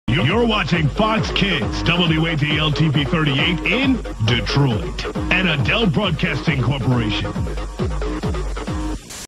WADL Station ID 1999
This is WADL's station ID from 1999
WADL is located in Mount Clemens, MI